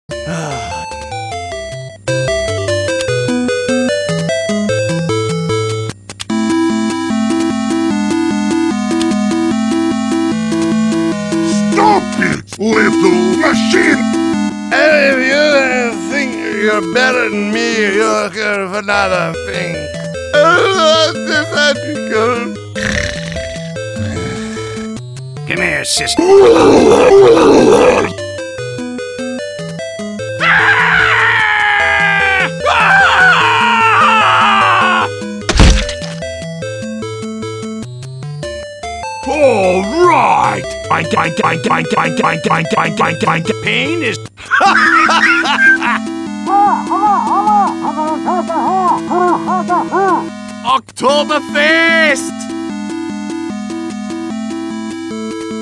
The red teams bar
Heavy plays tetris and Demoman has his private bar section